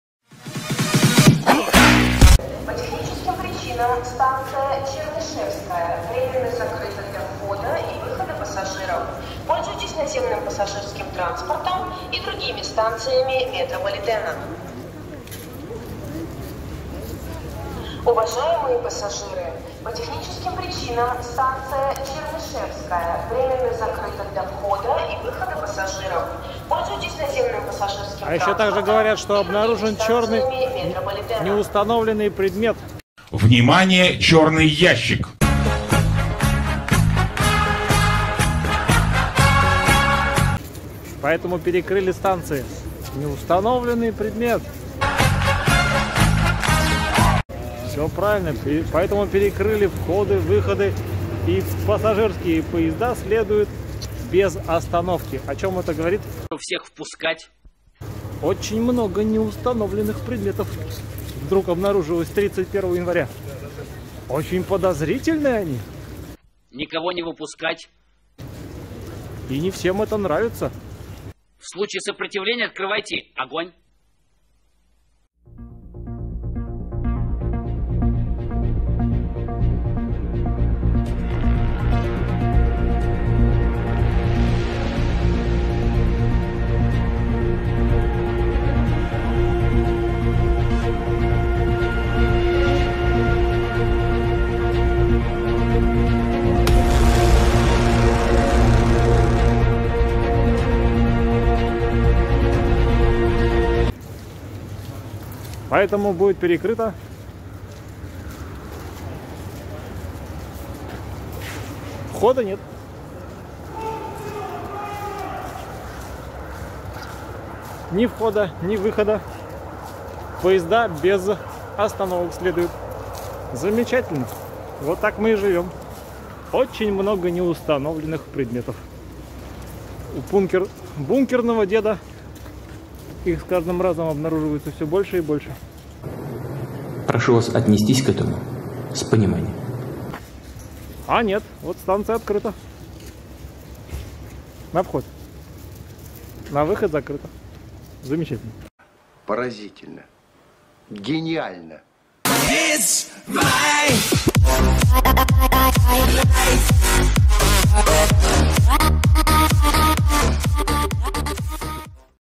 Почему закрыли метро в Питере.
31 января в Питере были массовые перекрытия в центре города, в том числе метро. По громкой связи объявляли о не установленном предмете. Но мы то с вами знаем - в КОМ тут дело!
   Властям давно плевать на нас, безопасность их интересует только в отношении себя.
   Сейчас была продемонстрирована, вскользь, готовность полиции применить табельное оружие - первый звоночек приближающейся неизбежной кровавой развязки.